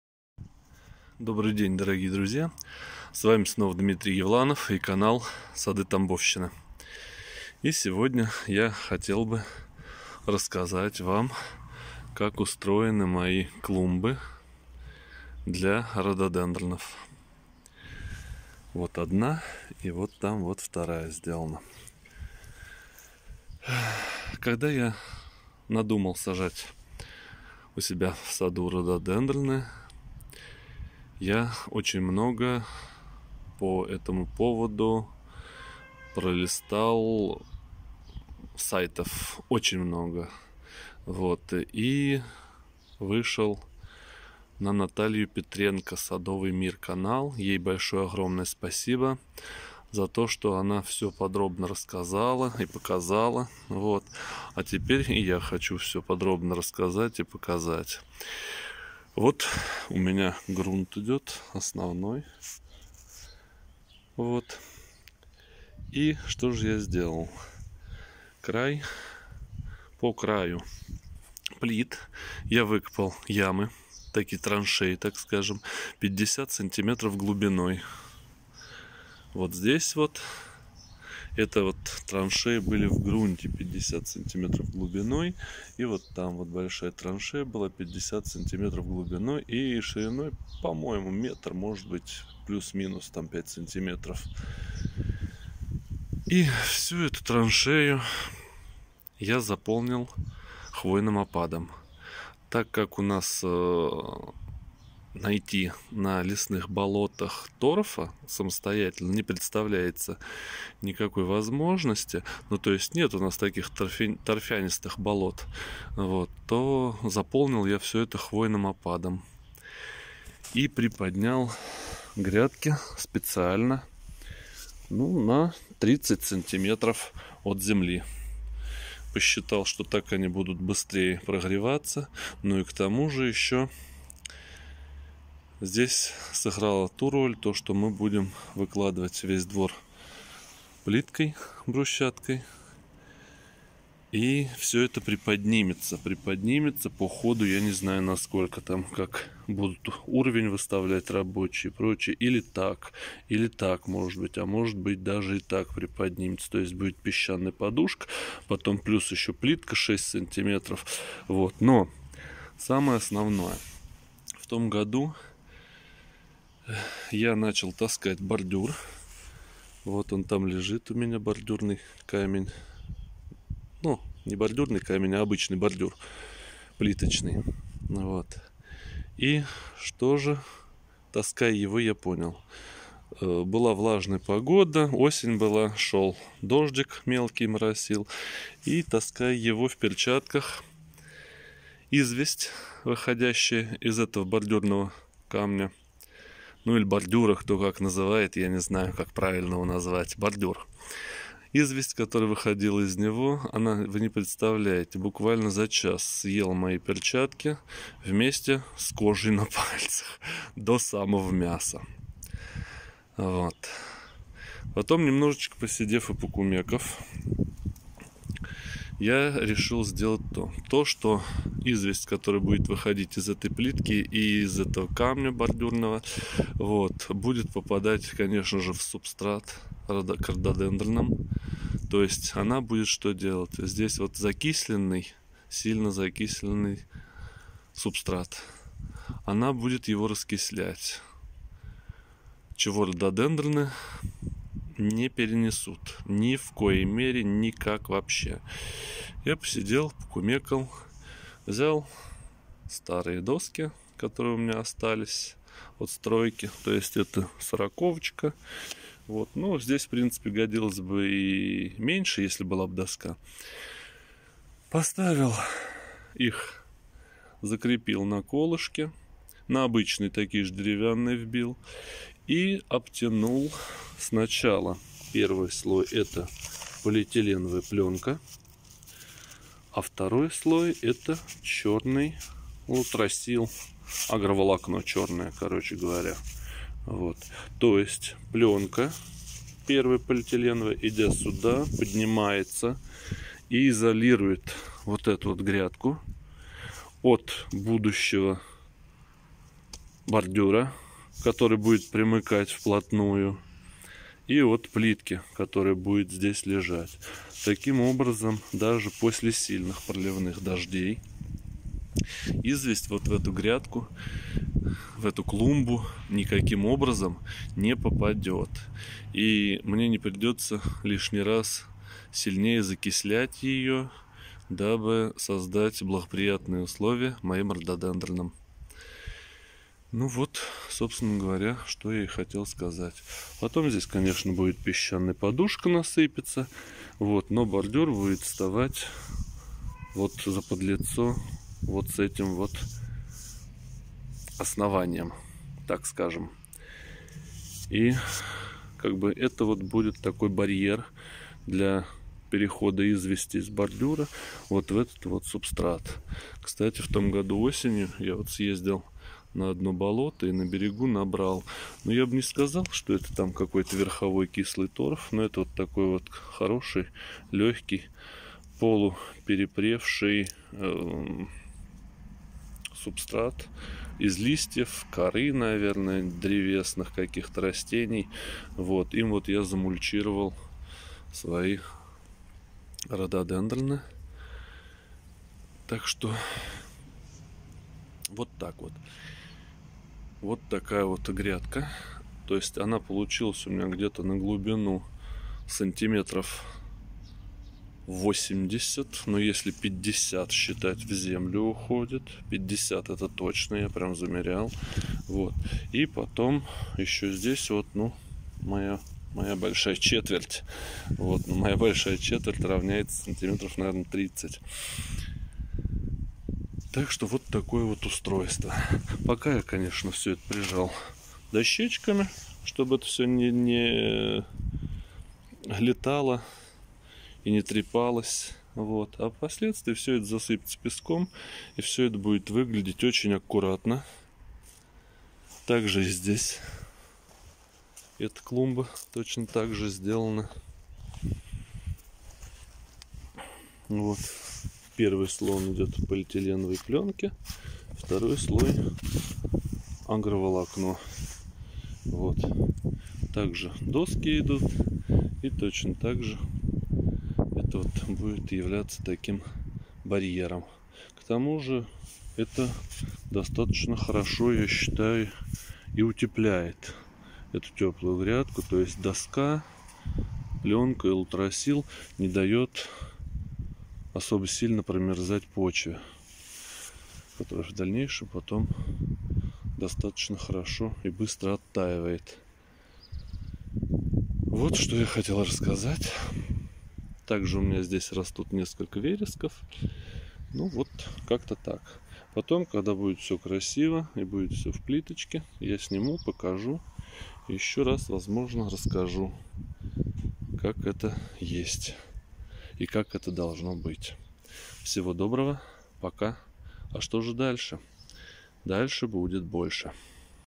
Моя клумба для рододендронов!
Как не дать кислому субстрату раскислится! Защита от внешних факторов!